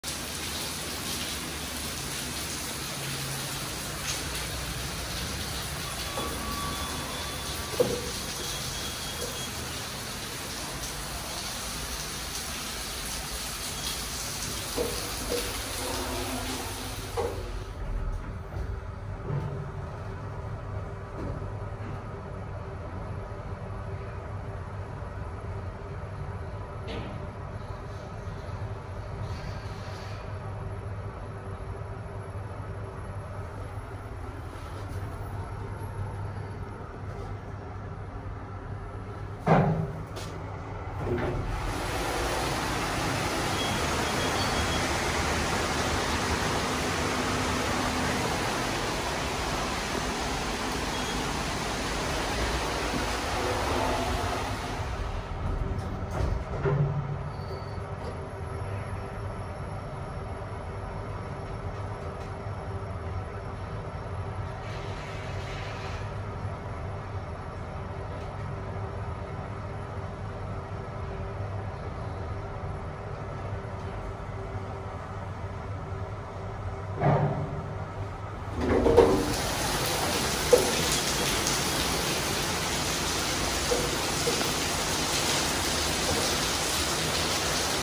KONE 3000 MONOSPACE MRL @ ENG HOON MANSIONS
Machine Room-Less (MRL) technology started taking off in Singapore in the early 2000s, with Kone being the first company to offer such a system, the Kone MonoSpace. With this possibility, many developers installed such units for low-rise developments or for retrofitting to existing shophouses. Such systems like these got so popular that Kone adapted the original MonoSpace to high-rise installations.